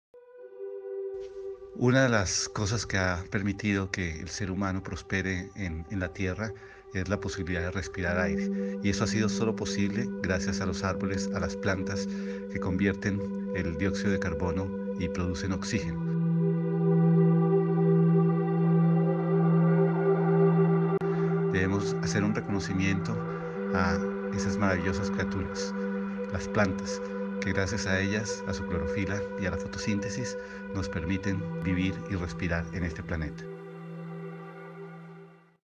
Día Interamericano de la calidad del aire
La Fundación Omacha, conmemora el Día Interamericano de la calidad del aire recordando la importancia que tienen los bosques para la conservación del aire.